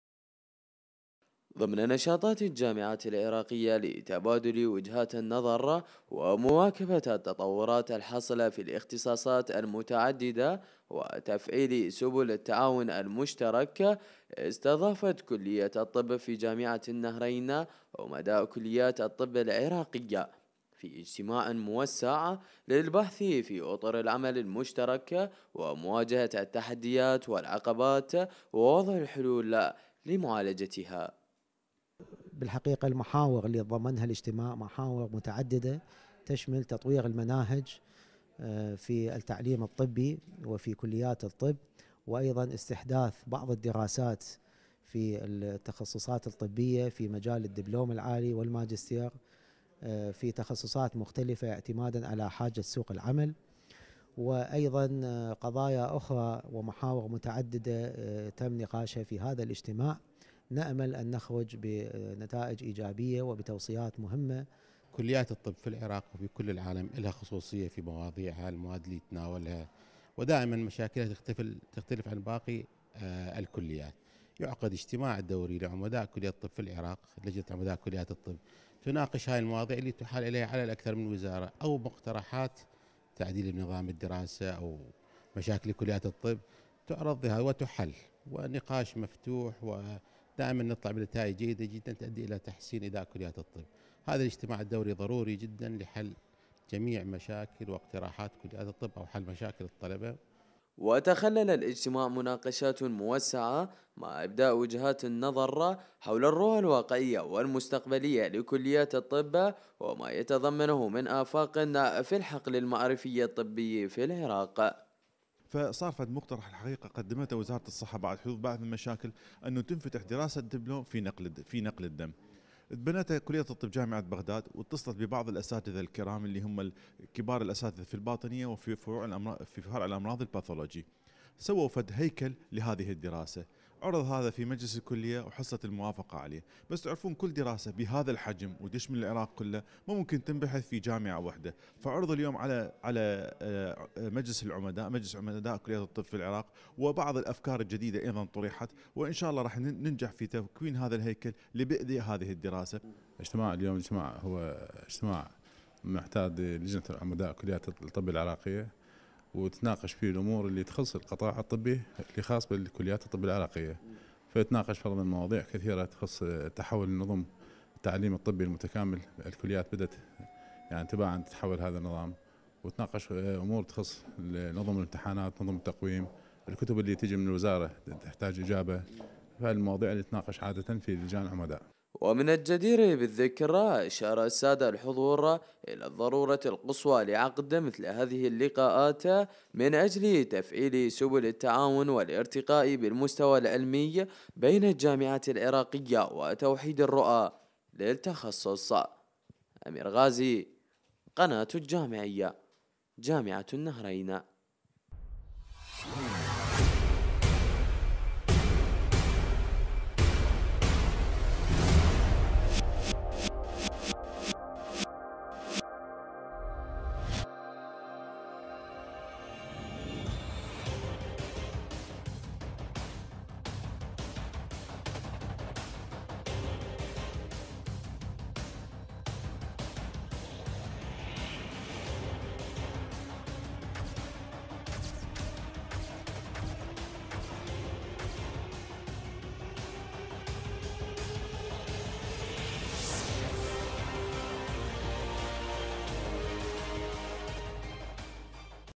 جامعة النهرين - اجتماع عمداء كلية الطب في العراق
تقرير من انتاج
مكتب القناة الجامعية الفضائية في جامعة النهرين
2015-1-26